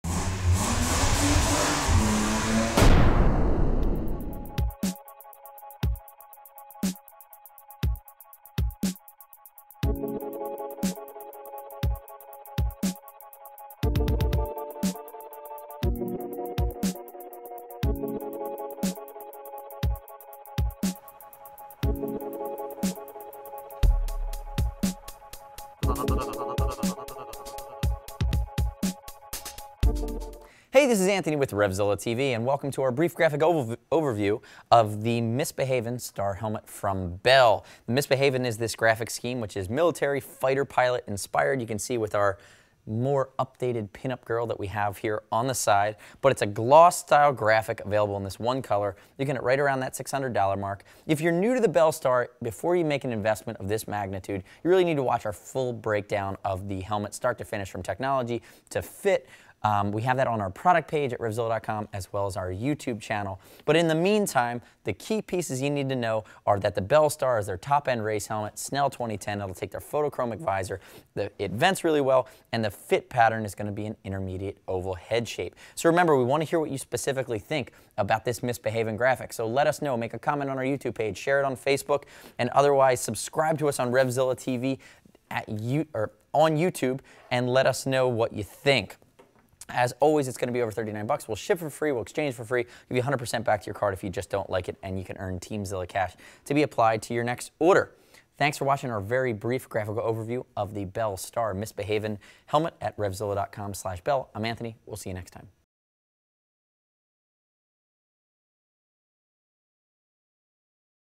Bell Star Miss Behavin Helmet Review at RevZilla.com
Bell Star Miss Behavin Helmet Review
The Bell Star Miss Behavin is not a graphic for those goody-two-shoes out there.  This is for the riders who like to bend the rules in their favor or who maybe are not above flirting their way out of a speeding ticket.  One color option on this helmet that features scantily clad ladies, dice and bullets.  What more do you need?  A full review of the Bell Star helmet can be viewed